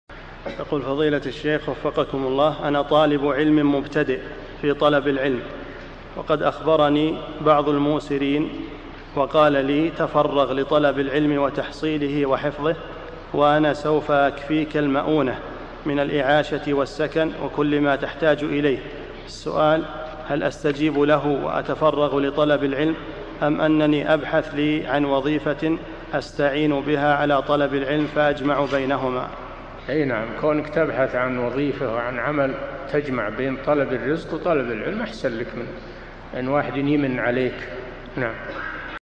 [698 -1024] قال لي أحد الموسرين: تفرغ لطلب العلم وأنا أكفيك المؤونة من الإعاشة والسكن، فما التوجيه؟
-[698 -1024] السؤال: أنا طالب علم مبتدئ في طلب العلم، وقد أخبرني بعض الموسرين وقال لي: تفرغ لطلب العلم وتحصيله وحفظه وأنا سوف أكفيك المؤونة من الإعاشة والسكن، وكل ما تحتاج إليه، السؤال: هل استجيب له وأتفرغ لطلب العلم، أم أنني أبحث عن وظيفة، استعين بها على طلب العلم فأجمع بينهما ؟
الجواب: إيه نعم كونك تبحث عن وظيفة وعن عمل فتجمع بين طلب الرزق وطلب العلم أحسن لك من أن واحدًا يمن عليك .
للاشتراك في قناة الشيخ صالح الفوزان
سؤال وجواب من شرح الفتوى الحموية ( 1024 ) مقطع

للاستفسار والإقتراحات والدعم
رئيس الفريق
لمشروع كبار العلماء